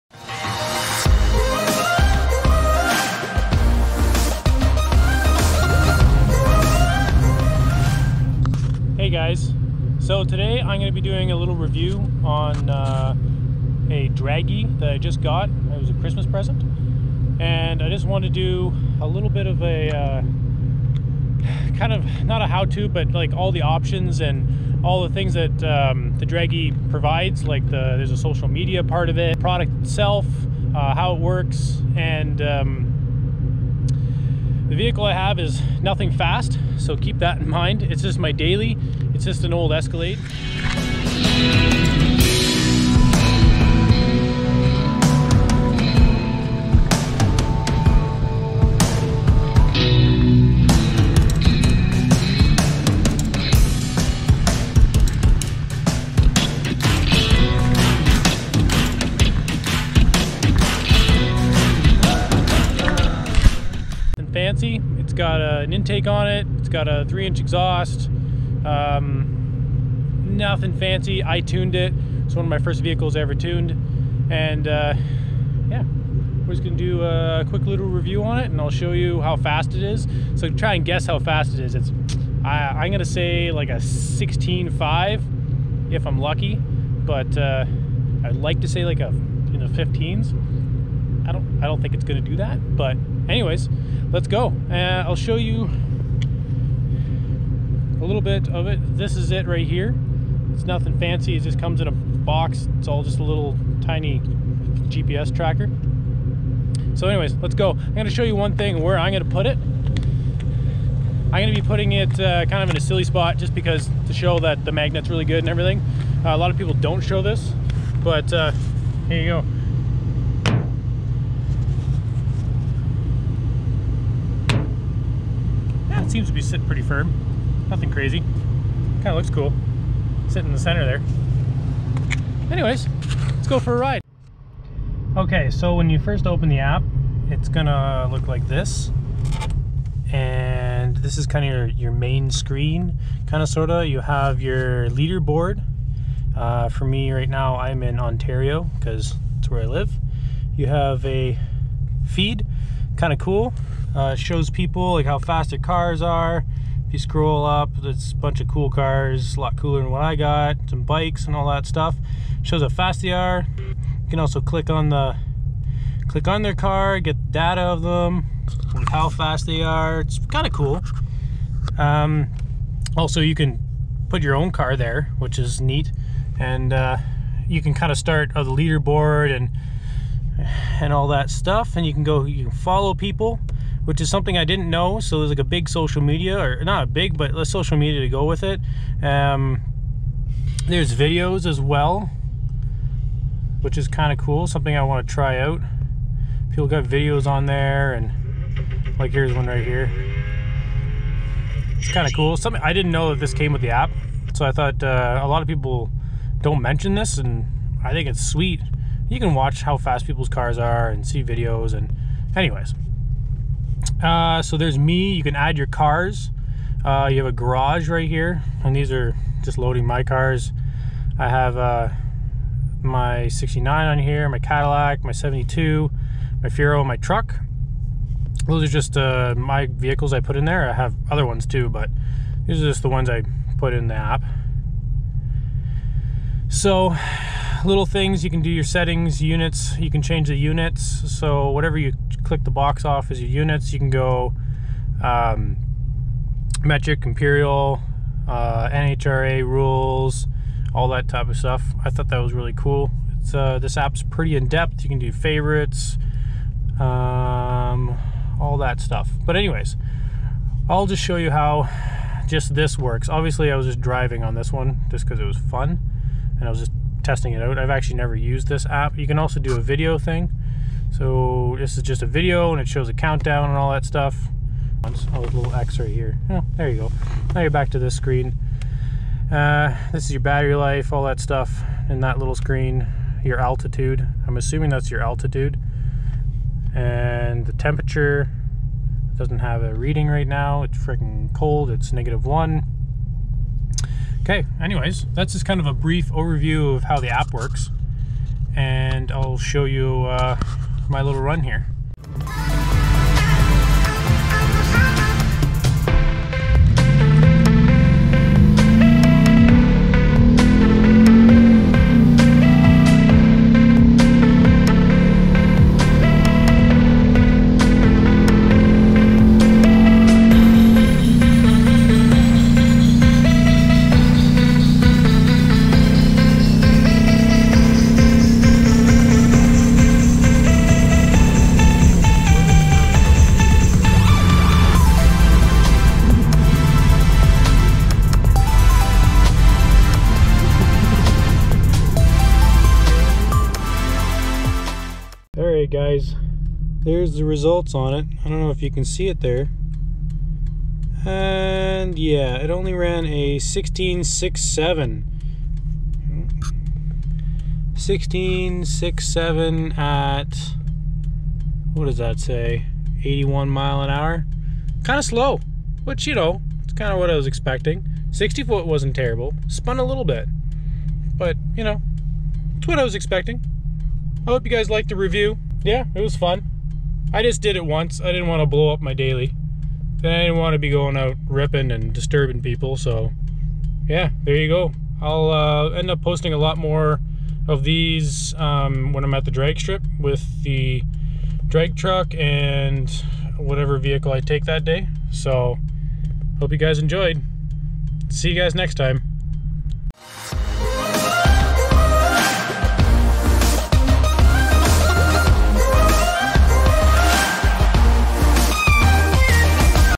Cadillac Escalade DRAGY Review
Here's our overview while trying out Dragy in the escalade for the first time! She did pretty good for an old girl!
60 ft 2.48s
330 ft 6.92s
1/8 mile 10.67s @ 65.8mph
1000 ft 13.91s @ 75.06mph
1/4 mile 16.67s @ 81.29mph